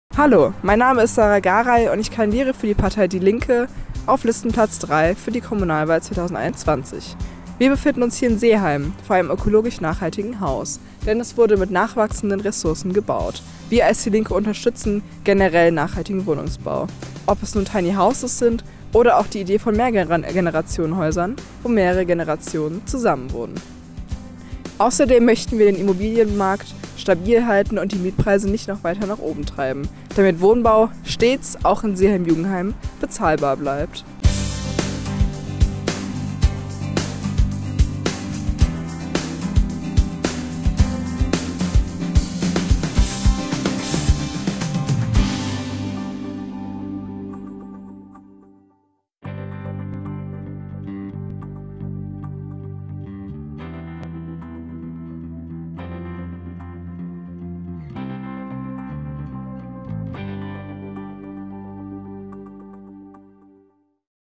Wohnen in Seeheim-Jugenheim
Auch in Seeheim-Jugenheim wird Wohnraum dringend benötigt. Dafür dürfen wir wenig neue Flächen nutzen und versiegeln und wollen lieber vorhandenen Wohnraum nachverdichten, über Supermärkten bauen oder alternative Wohnformen wie Tiny Houses oder Mehrgenerationenhäuser fördern.

◄ DIE LINKE. Seeheim-Jugenheim auf Facebook